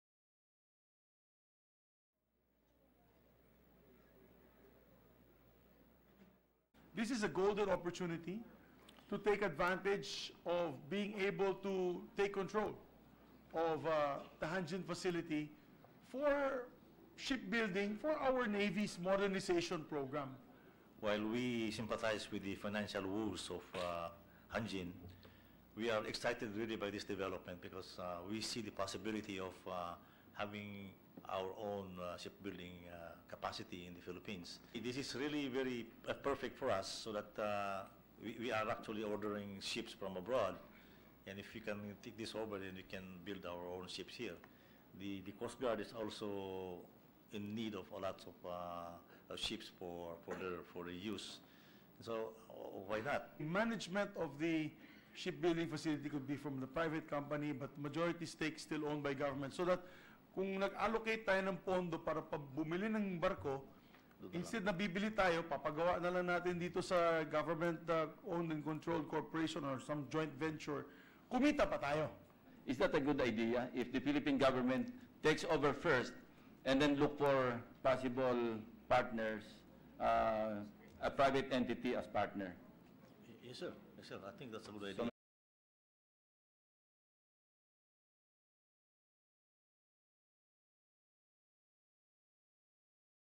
Government takeover of Hanjin eyed
The government can take over the shipyard of Hanjin Heavy Industries and Construction Corp. Philippines which has declared bankruptcy, Senate Majority Leader Juan Miguel Zubiri proposed Wednesday.